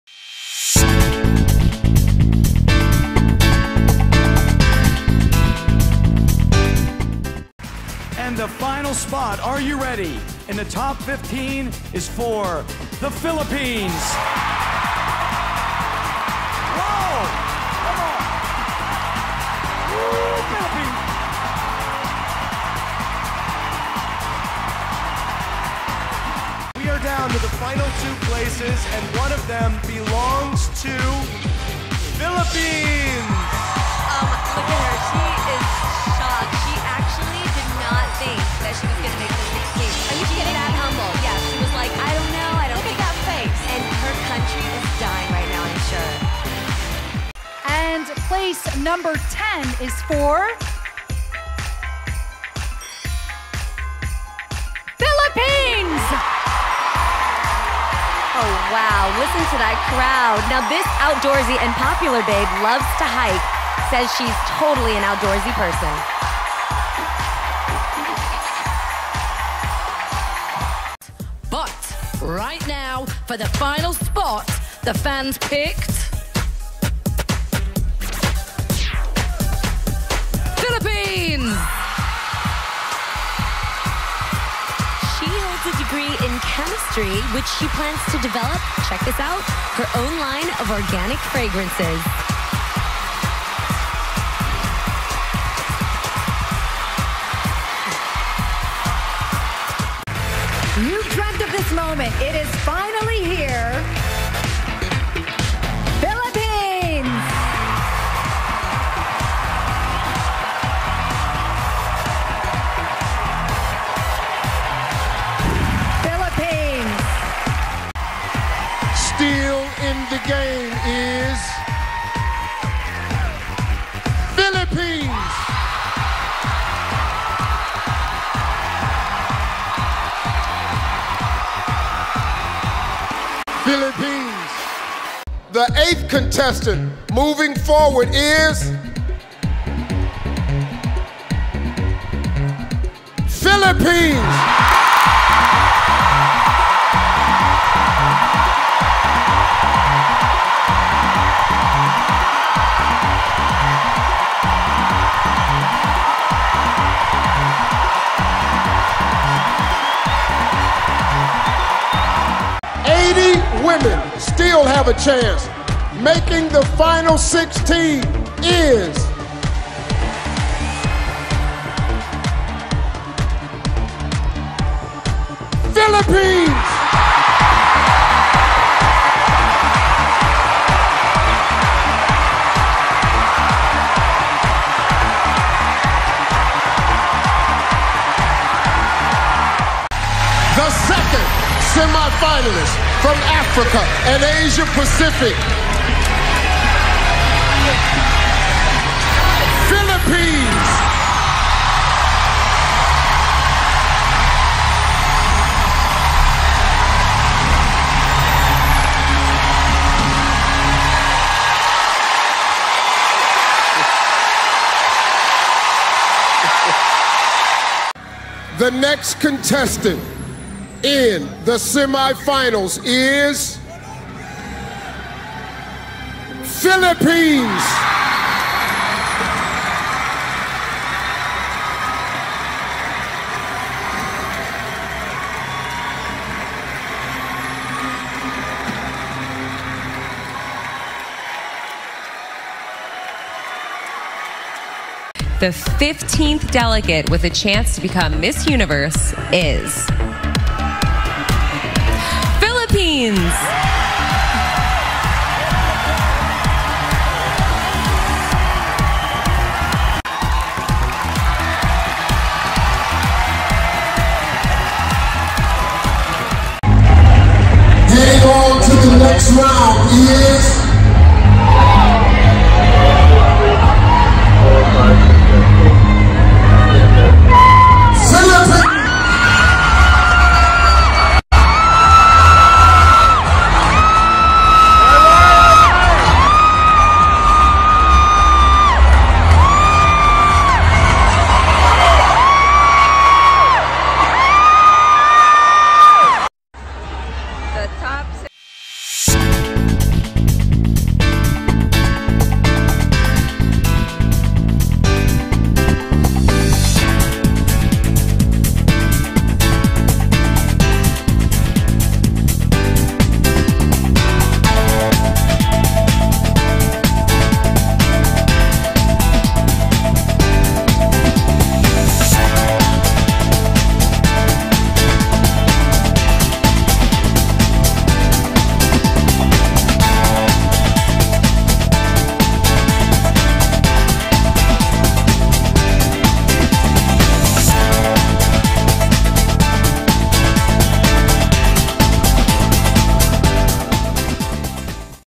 Philippines 12-Year Streak in Miss Universe | Longest Active Placement Streak
The Philippines has surpassed India in terms of Miss Universe semifinal placement streak and now also holds the longest active streak after Beatrice Luigi Gomez delivered a strong Top 5 finish at the Miss Universe 2021 pageant held in Israel.

In the past, only two countries have achieved a historic two-decade placement streak, USA and Venezuela, but the Philippines has consistently sent strong and well-prepared candidates over the last decade and is poised to achieve several more in the years to come.

Will the Philippines achieve a 20-year streak and complete the powerhouse trifecta of Miss Universe?

2010 - Venus Raj, 4th Runner Up
2011 - Shamcey Supsup, 3rd Runner Up
2012 - Janine Tugonon, 1st Runner Up
2013 - Ariella Arida, 3rd Runner Up
2014 - Mary Jean Lastimosa, Top 10
2015 - Pia Wurtzbach, WINNER
2016 - Maxine Medina, Top 6
2017 - Rachel Peters, Top 10
2018 - Catriona Gray, WINNER
2019 - Gazini Ganados, Top 20
2020 - Rabiya Mateo, Top 21
2021 - Beatrice Luigi Gomez, Top 5